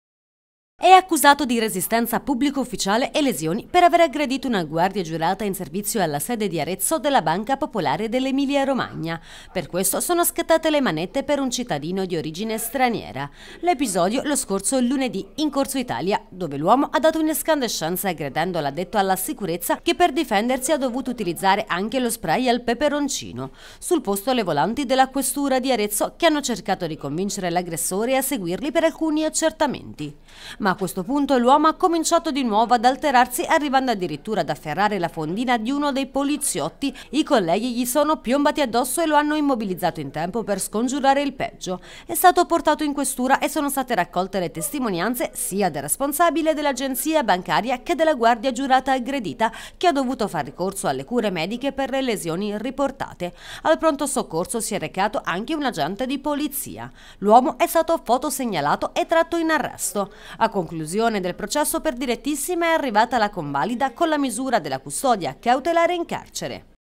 Aggredisce guardia giurata della banca e poliziotti: arrestato per resistenza e lesioni
E' accusato di resistenza a pubblico ufficiale e lesioni per aver aggredito una guardia giurata in servizio alla sede di Arezzo della Banca Popolare dell'Emilia Romagna.
Per questo sono scattate le manette per un cittadino di origine straniera.